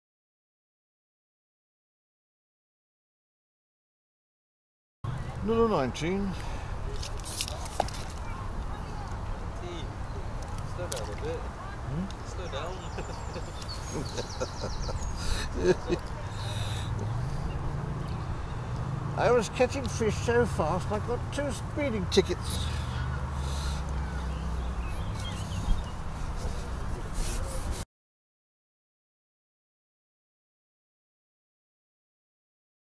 Speed Fishing
Short video, James is back, nice to get out fishing with the boy.